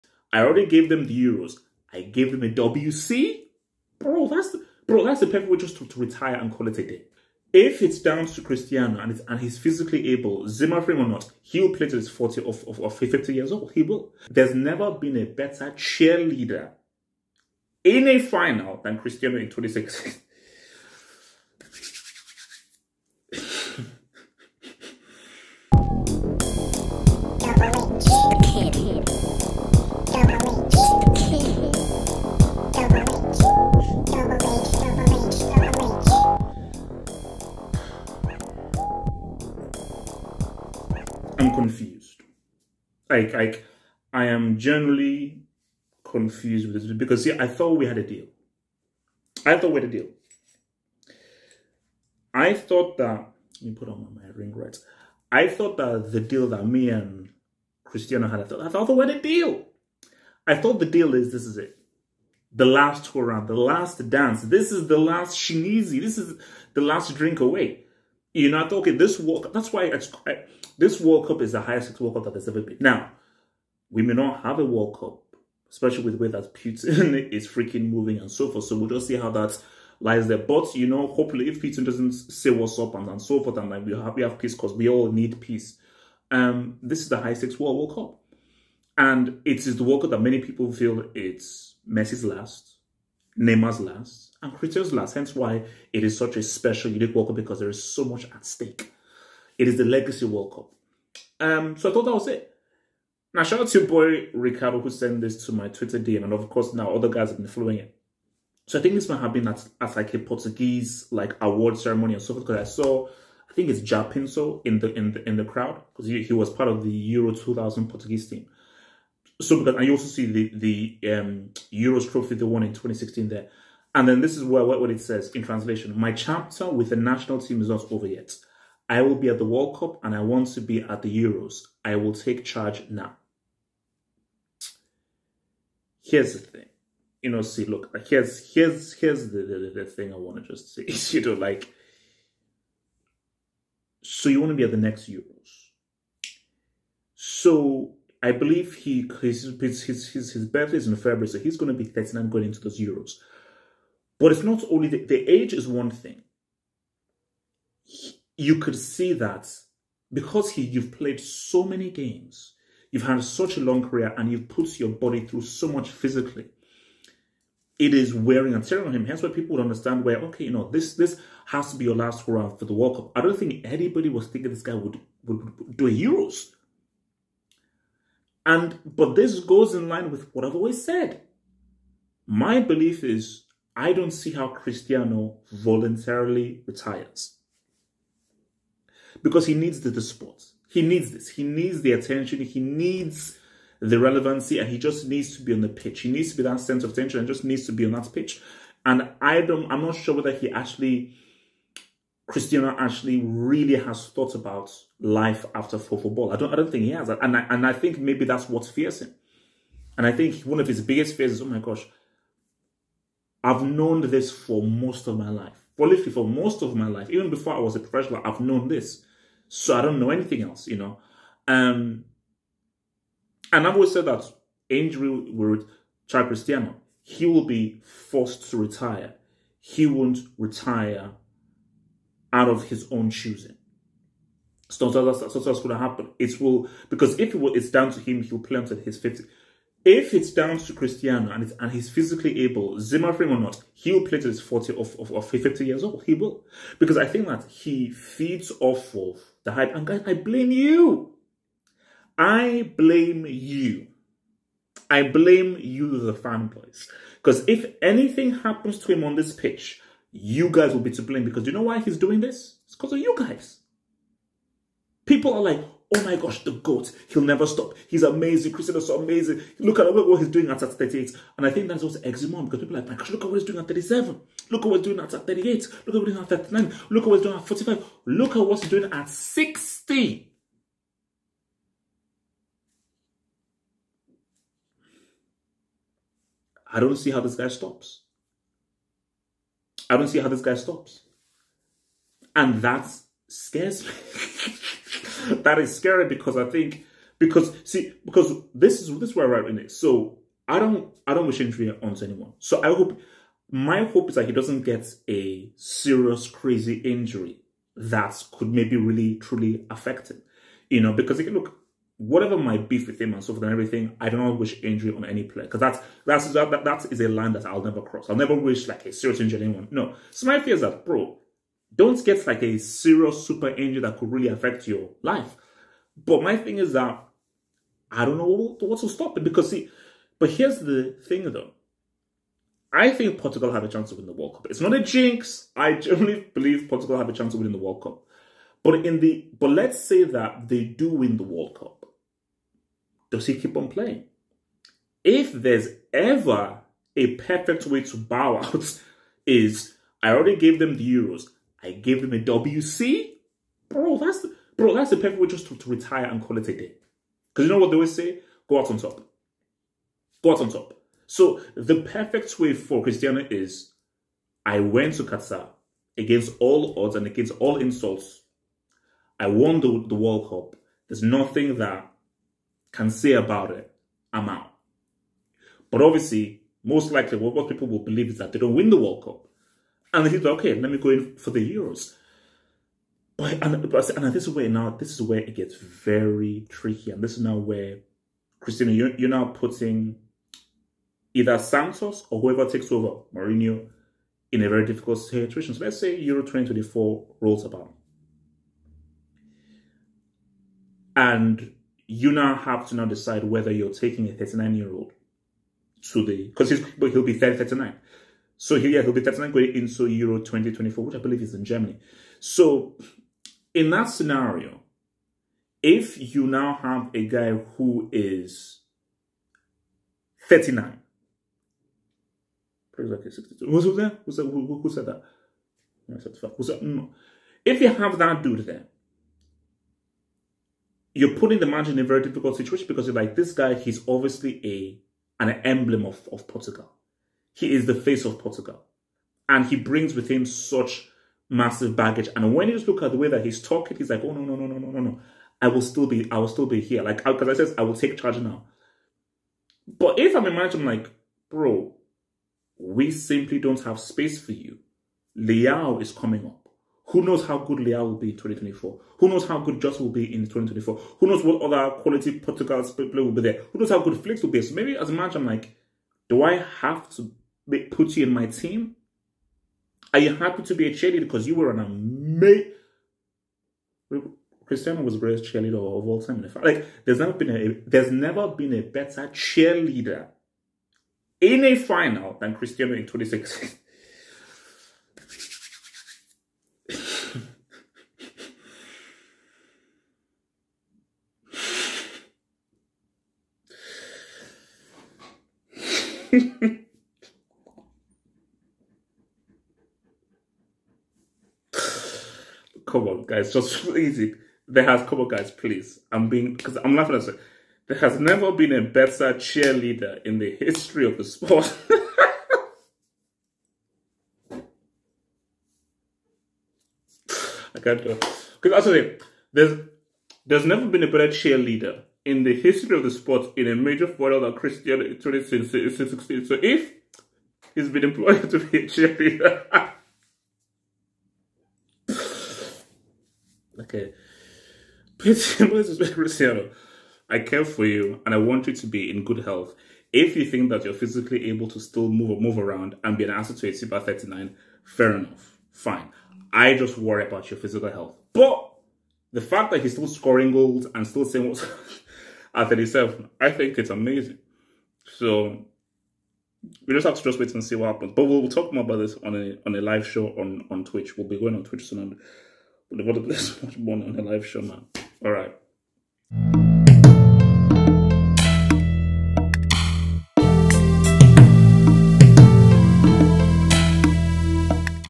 Ronaldo at Euro 2024?!! Says Qatar World Cup WON'T Be His Last. He's Playing Till He's 60!
Cristiano Ronaldo says the Qatar World Cup won't be his last for Portugal. Manchester United striker wants to play in Euro 2024. Have Hope gives his reaction